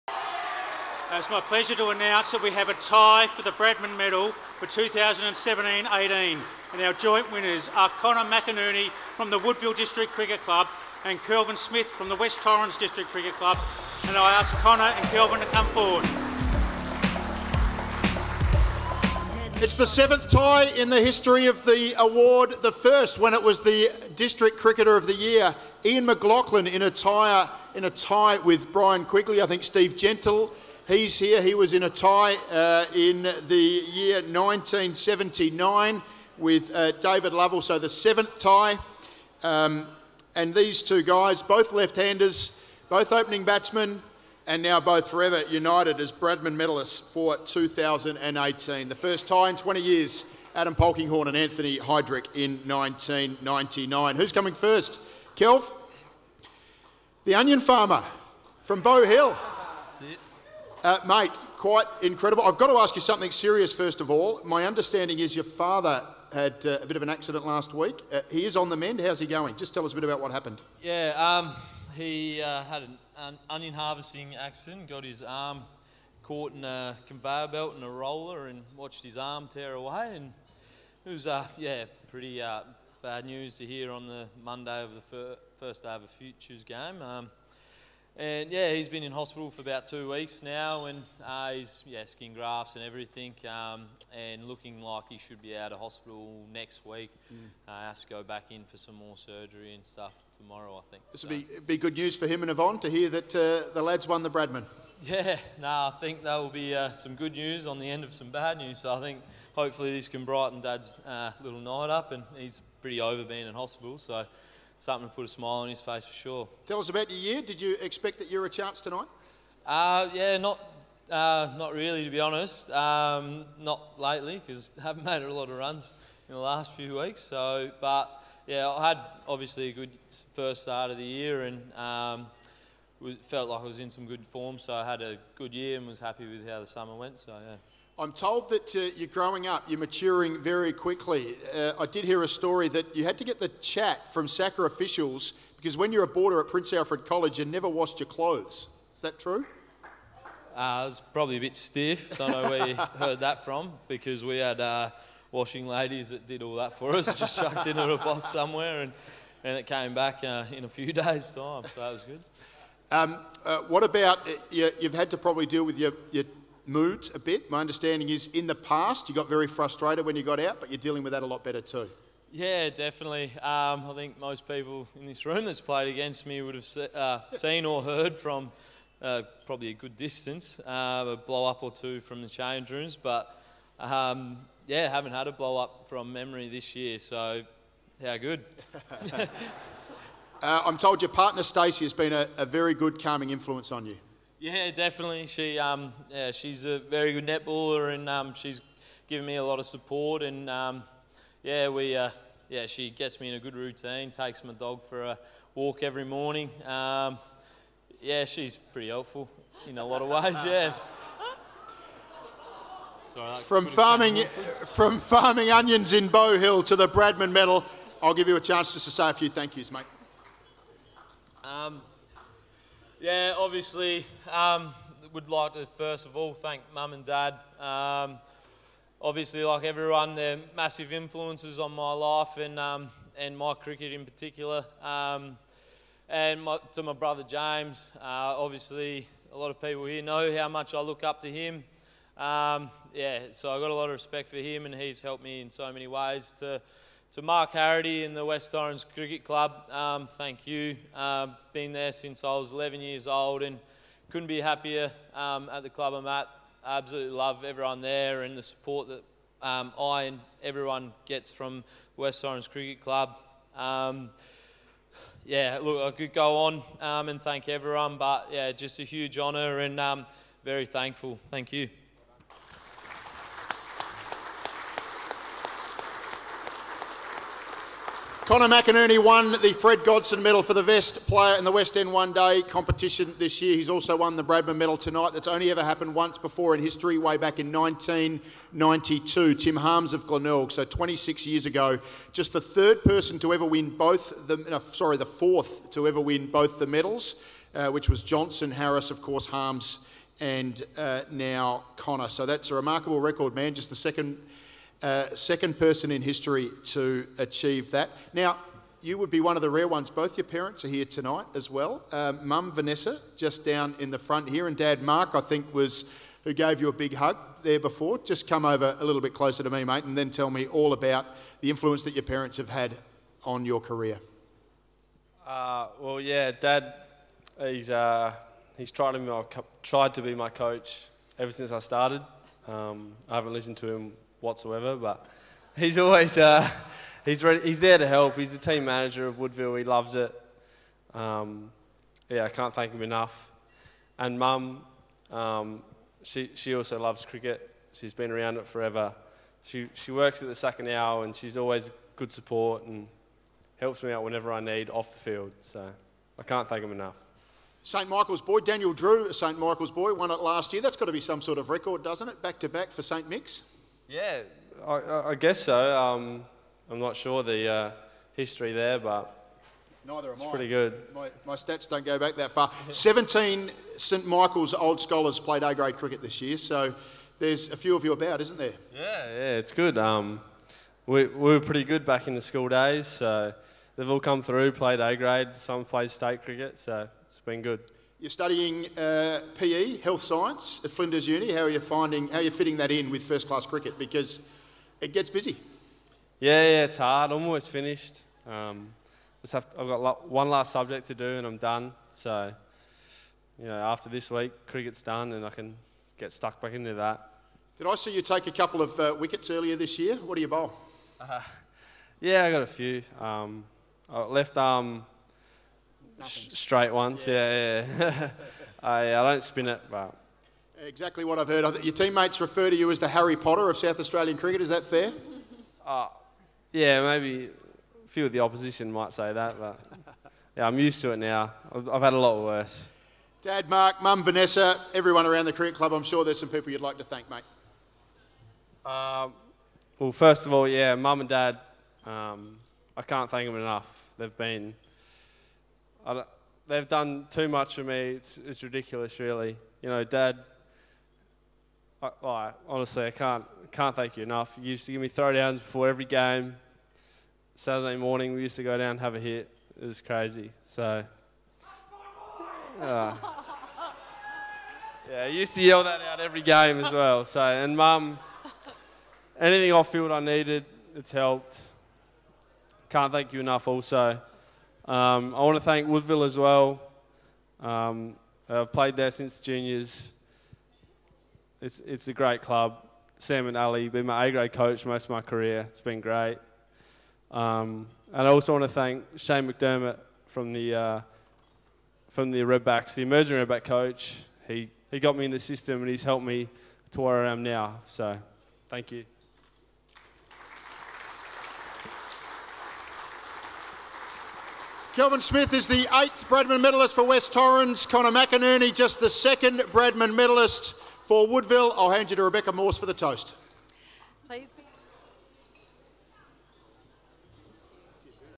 Smith, McInerney declared Bradman Medallists
Kelvin Smith of West Torrens and Conor McInerney of Woodville couldn't be split at the 60th Bradman Medal count at Adelaide Oval.